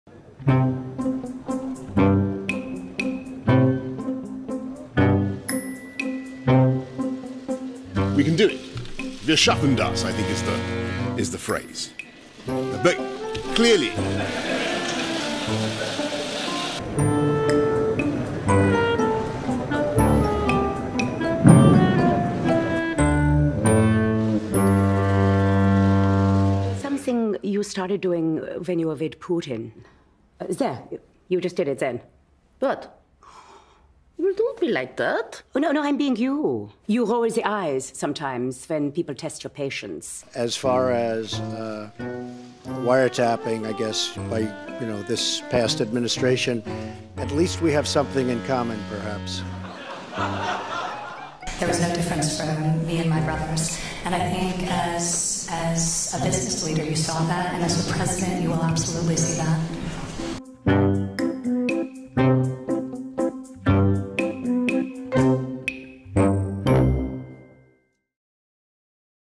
How Merkel's eye rolls betray her true feelings on allies
German Chancellor Angela Merkel famously rolled her eyes at Putin during a G20 meeting. She's since shown her reactions betray her true feelings for her allies, after doing the same with the Trumps and Boris Johnson.